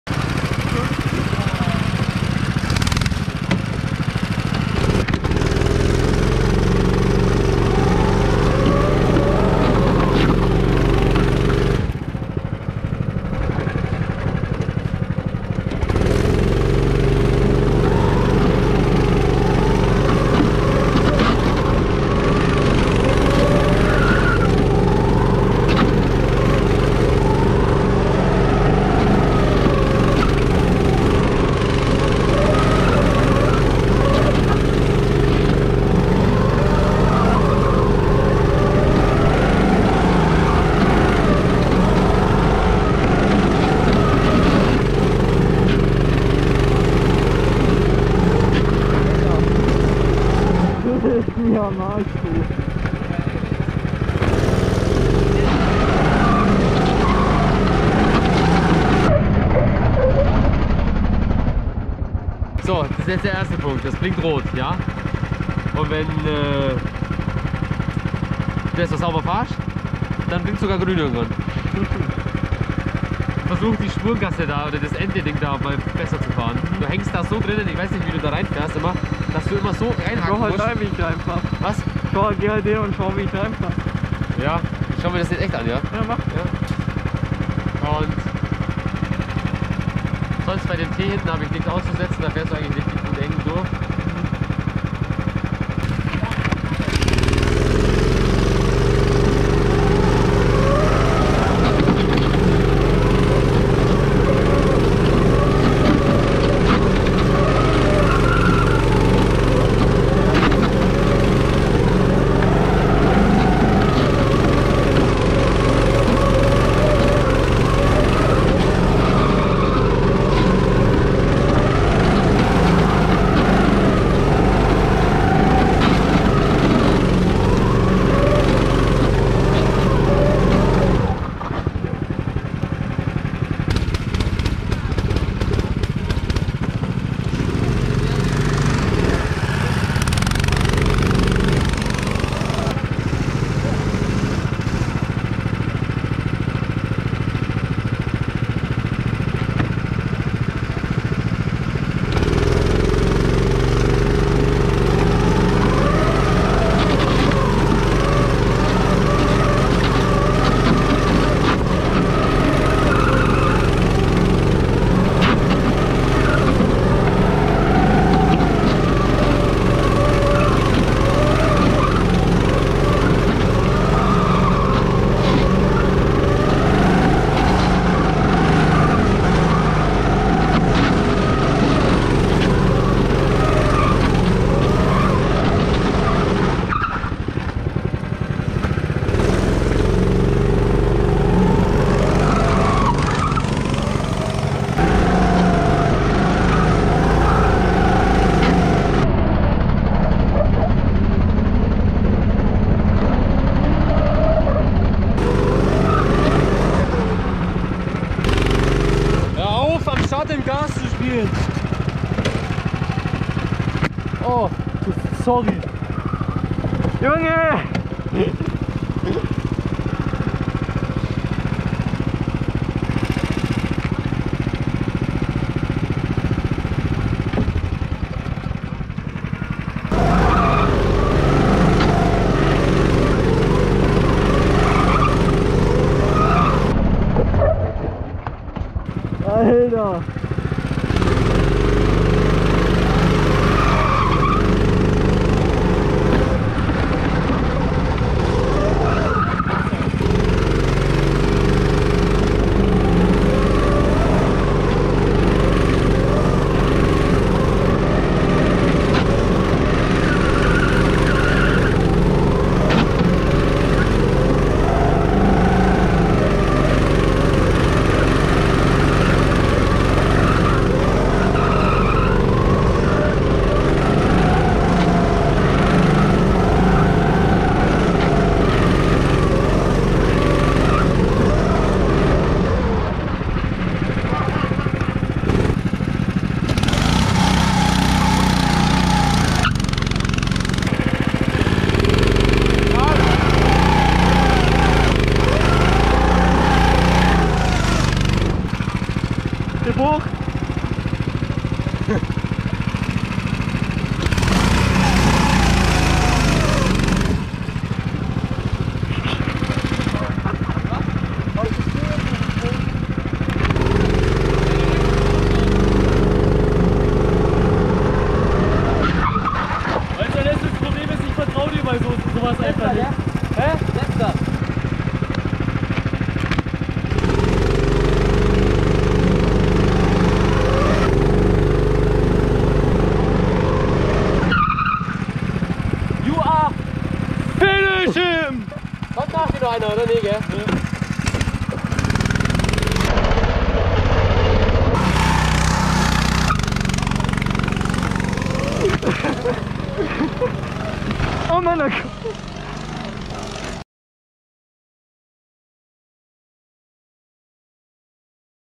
Racing beginnt wieder! (Lustiges Training😂)
Mit meiner neuen GoPro beginnt auch wieder die neue Go-Kart Saison!

Damit habe ich gefilmt:

Kamera Equipment:
Action Cam

Mein Discord
Azzykill#9135

Social Medias📱

azzykillyoutube

Azzykill